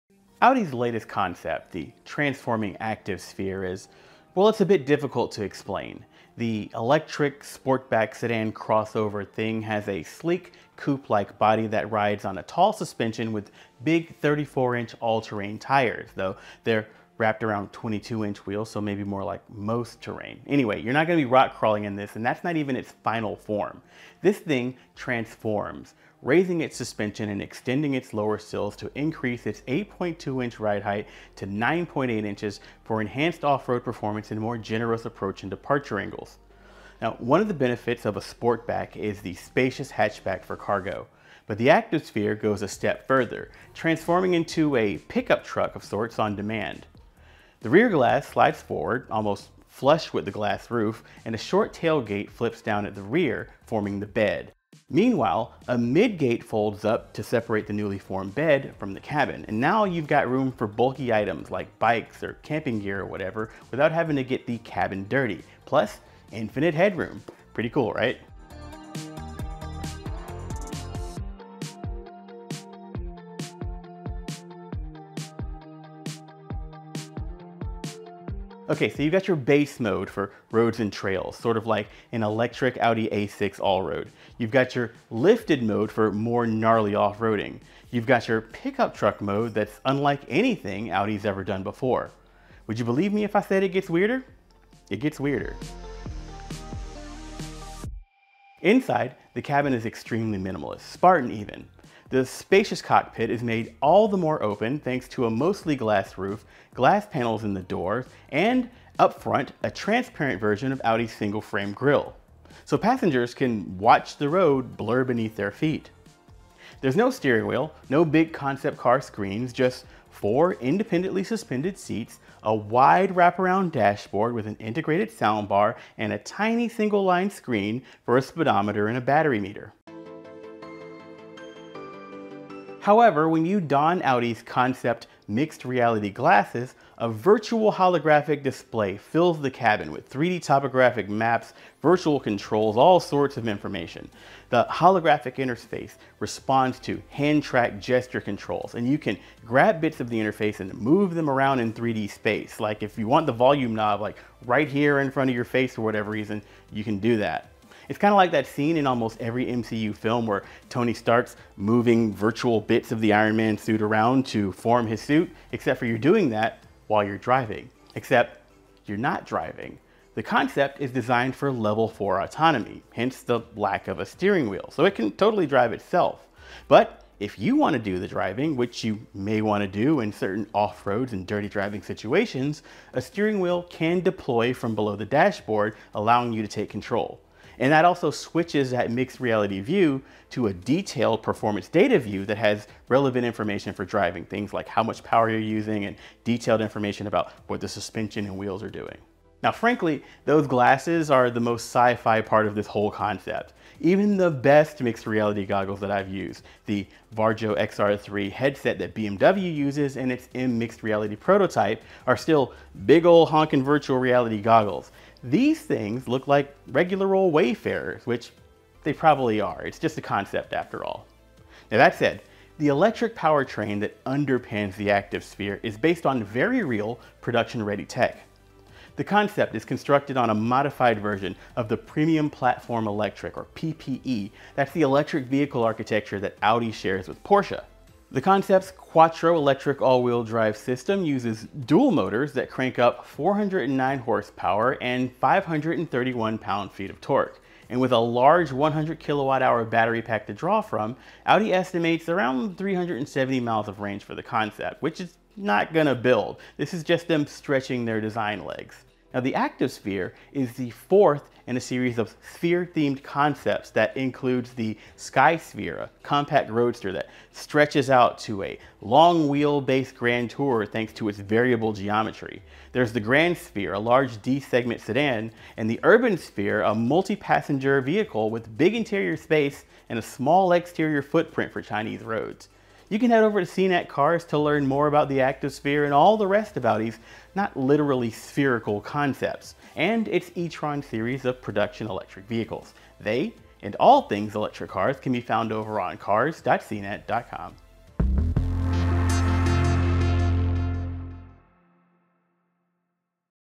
Your Future Car Could Transform Into a Pickup Truck – The Audi Activesphere
Audi's new concept battery electric coupe reshapes its body on-demand for improved off-road utility and reimagines the cabin with augmented reality inspired by science fiction.

0:00 Audi Activesphere
1:18 Driving Modes
1:38 Interior Design
2:09 Audi Mixed Reality Glasses and Autonomous Driving
3:40 Powertrain Electric Architecture
4:18 Audi Concept Cars, Skysphere, Grandsphere, Urbansphere